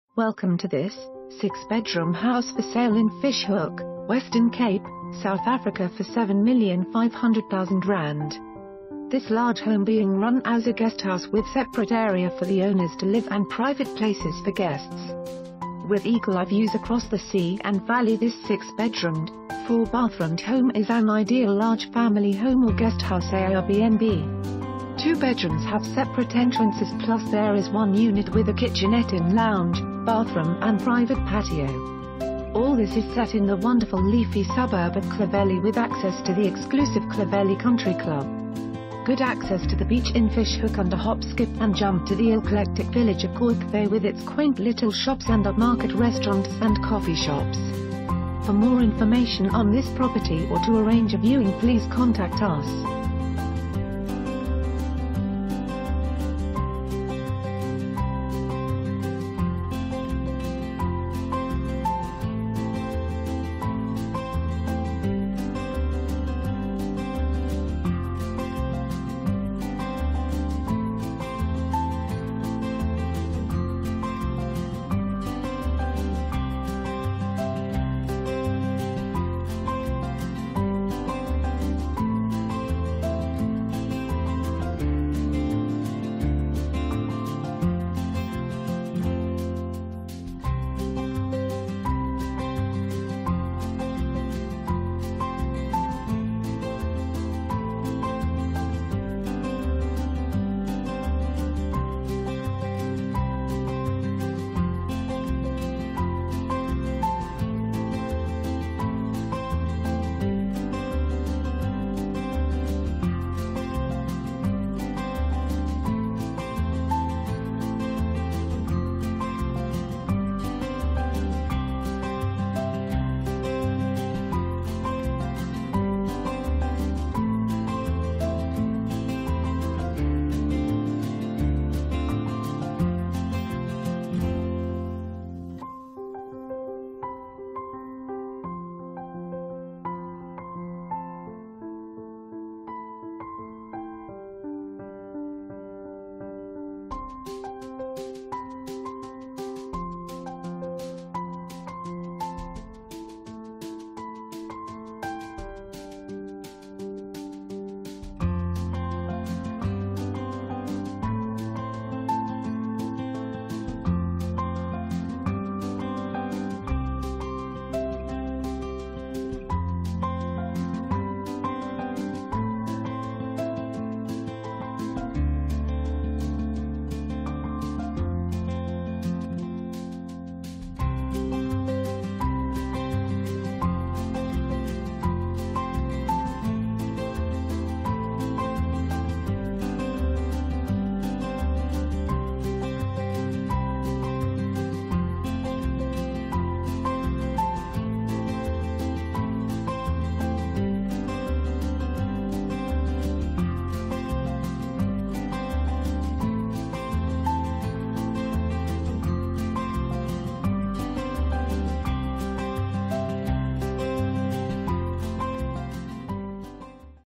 6 Bedroom House For Sale in Fish Hoek, Western Cape, South Africa for ZAR 7,500,000
This large home being run as a guesthouse with separate area for the owners to live and private places for guests. With eagle eye views across the sea and valley this six bedroomed, four bathroomed home is an ideal large family home or guesthouse/Airbnb. Two bedrooms have separate entrances plus there is one unit with a kitch...